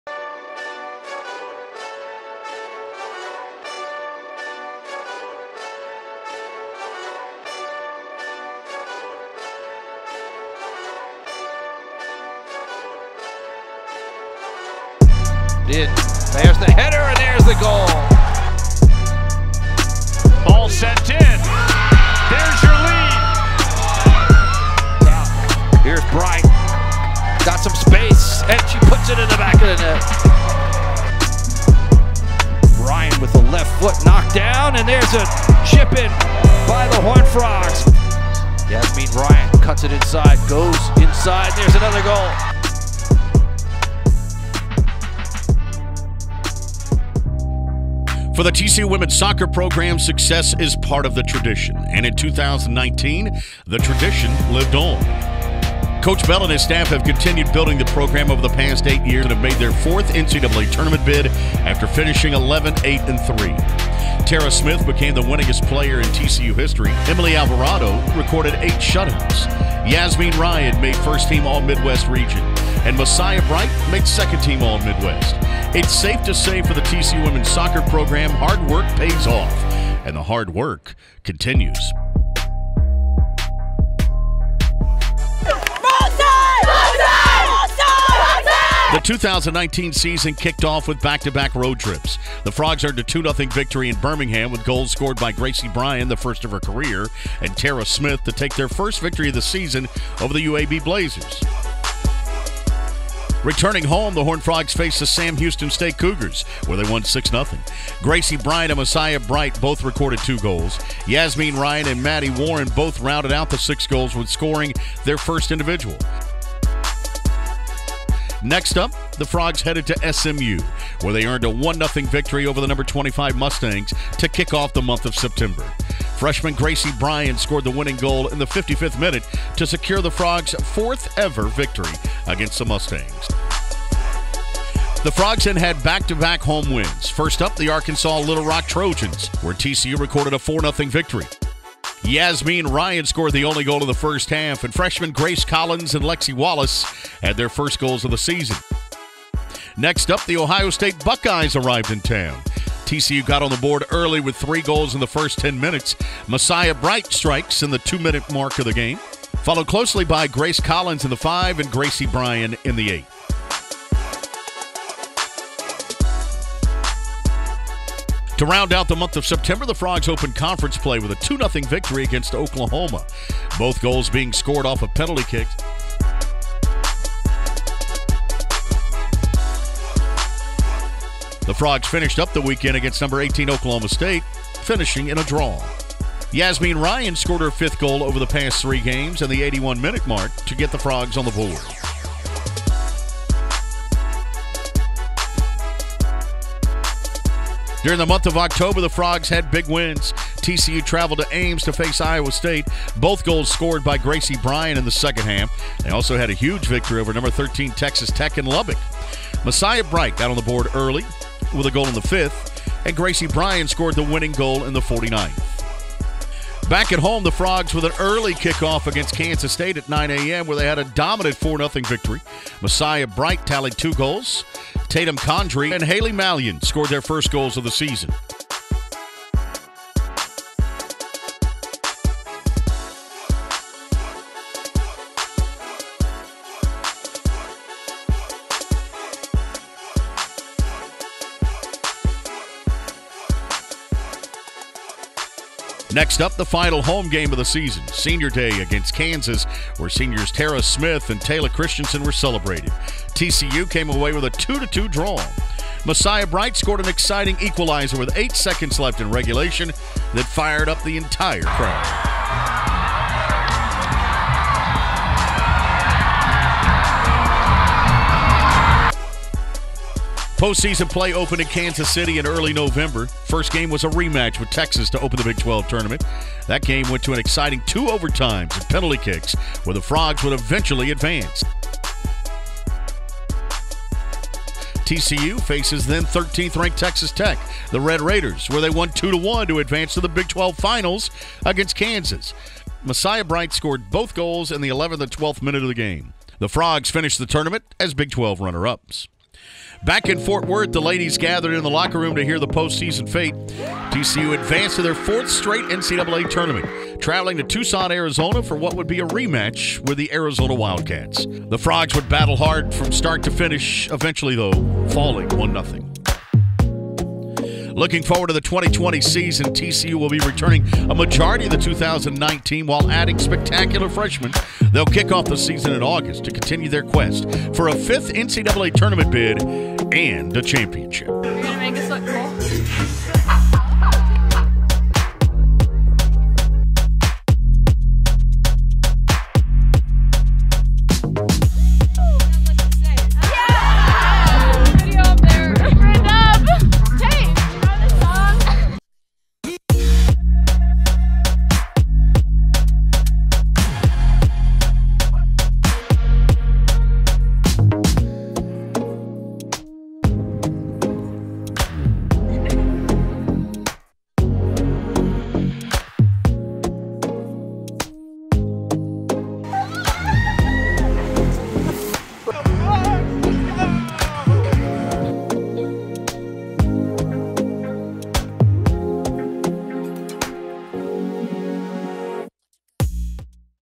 TCU Soccer 2019 Year in Review
Highlights from the 2019 TCU Women's Soccer season as the Horned Frogs earned their fourth-straight NCAA Tournament berth.